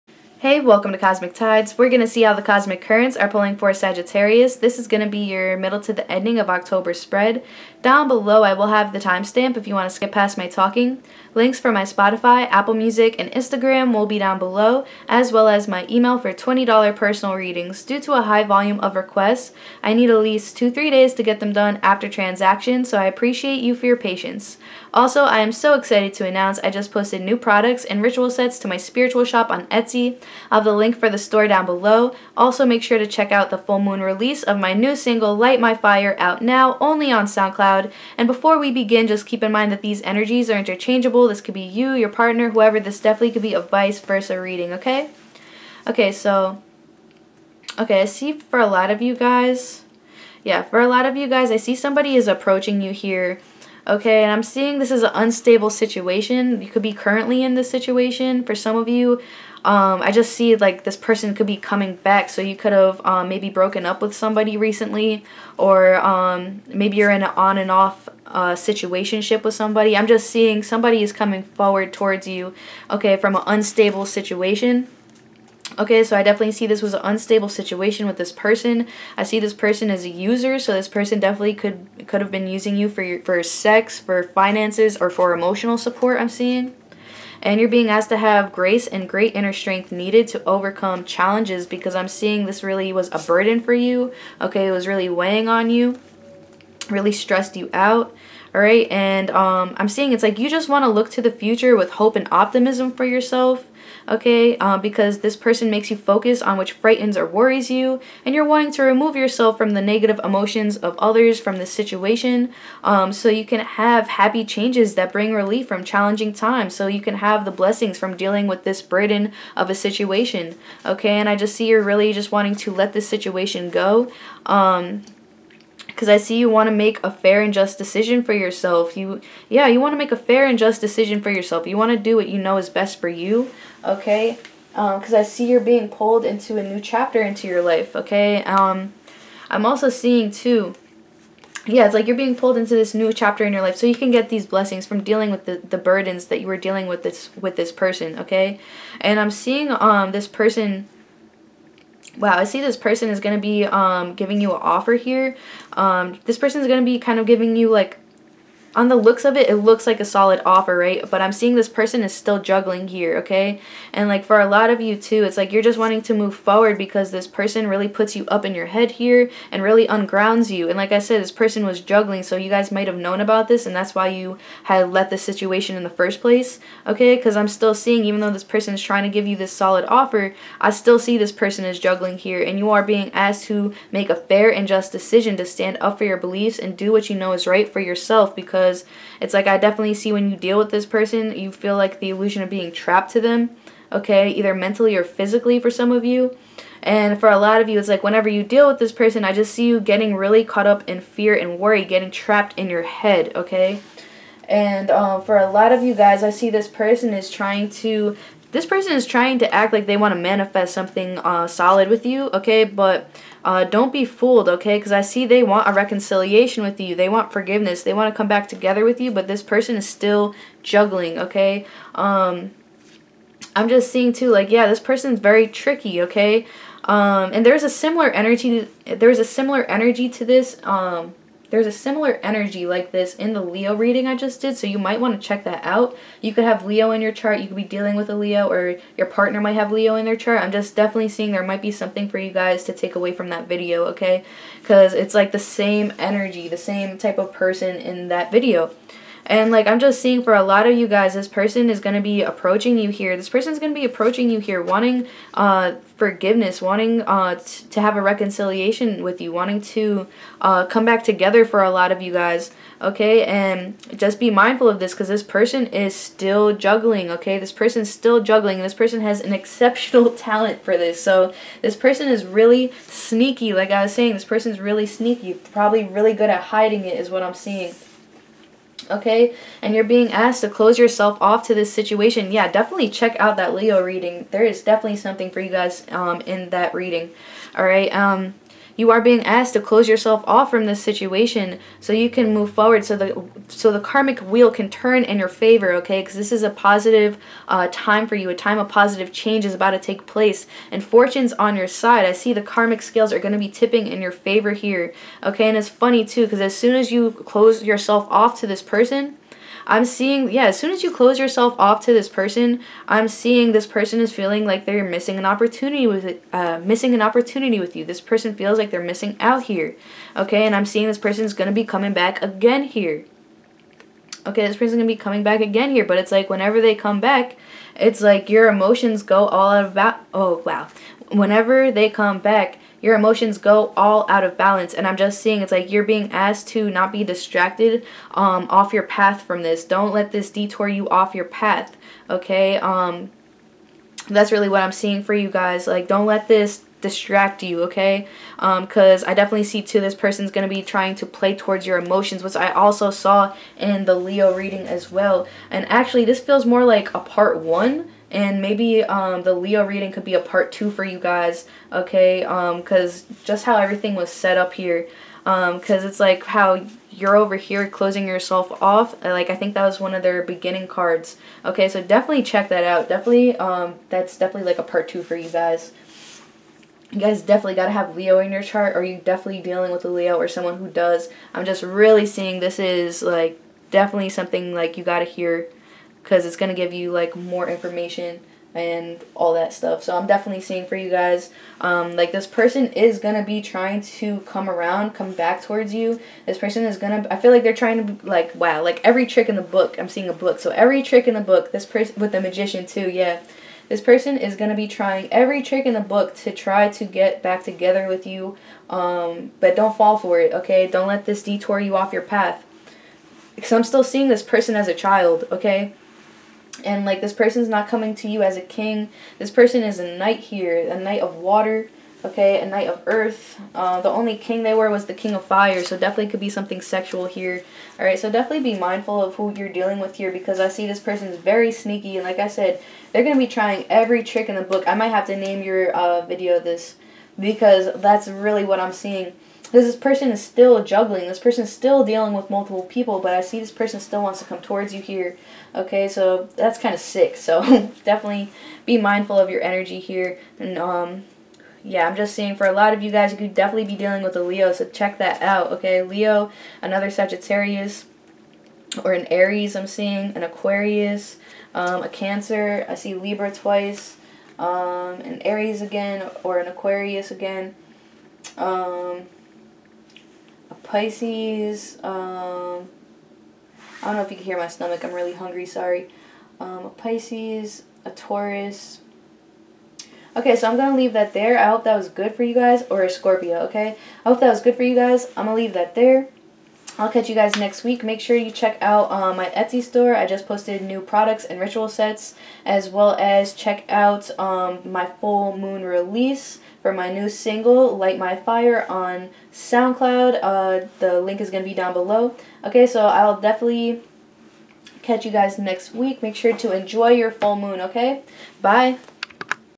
Sagittarius Mid-End October, THEY'RE GONNA USE EVERY TRICK IN THE BOOK--DON'T FALL FOR IT
Intuitive reading for Sagittarius, from the middle to end of October. I stop talking at 0:25

-DEFINITELY CHECK OUT THE LEO MID-END OCTOBER AND BONUS CAPRICORN SINGLES READING, MIGHT BE DEALING WITH OR HAVE IN YOUR/PARTNERS CHART.-

--THE NUMBER 818 COULD ALSO MEAN SOMETHING FOR SOME OF YOU--

Soundcloud.com/vic_music